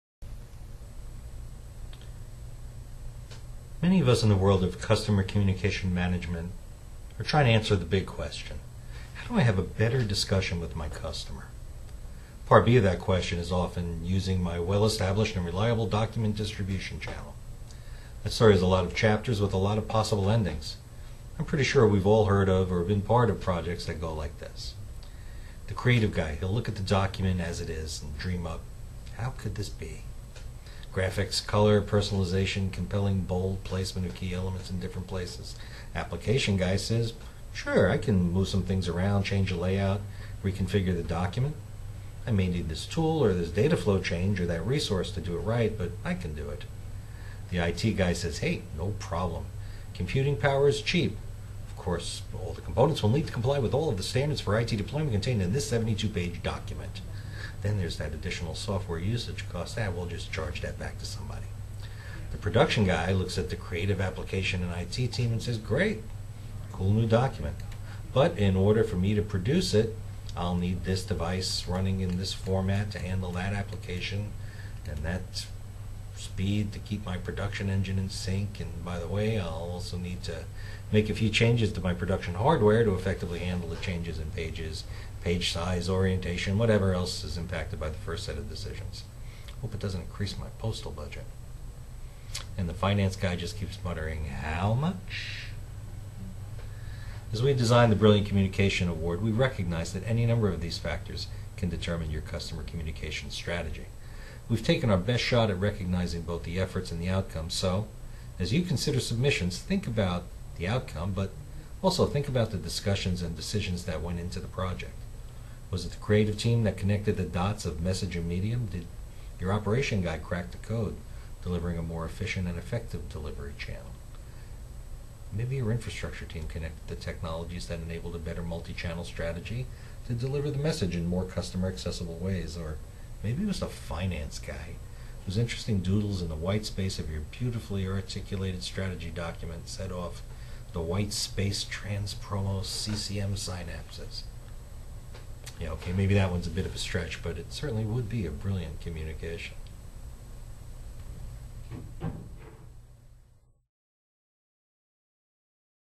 Pitney Bowes: How to Have a Better Conversation with Your Customers
Pitney Bowes' Michael Calderwood offers insights on "brilliant communications," and wisdom on how to have a better conversation with customers